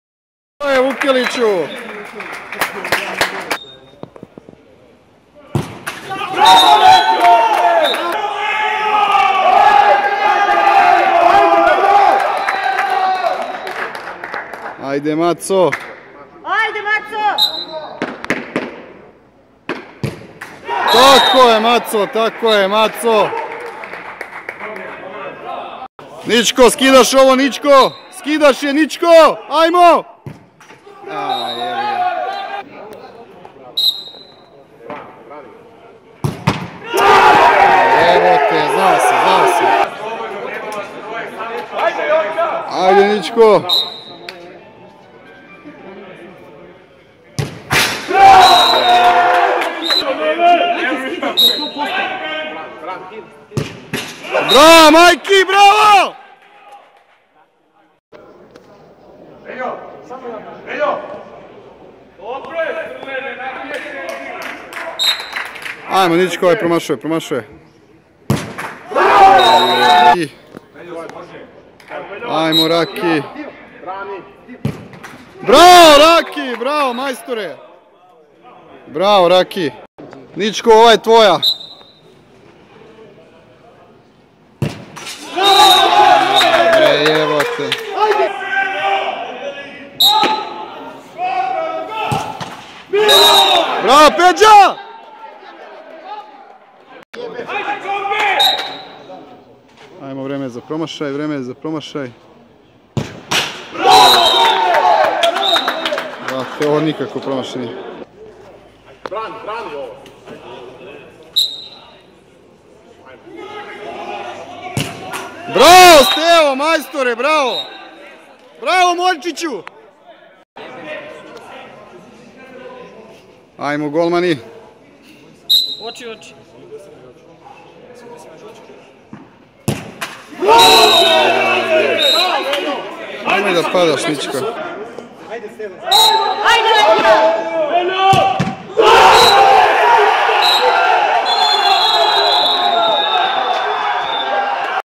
Nova Rent - Baranda 6:7 penali
Legija promašuje odlučujući penal